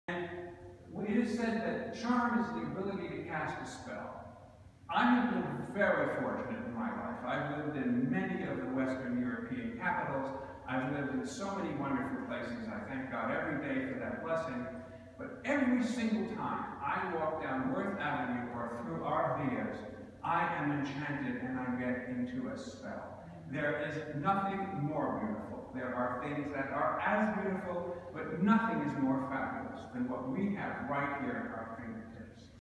Rene Silvin at the Fellowship of Christians and Jews exploring the Palm Beach Vias
Rene describes why he loves the Vias in Palm Beach, which were developed by Addison Mizner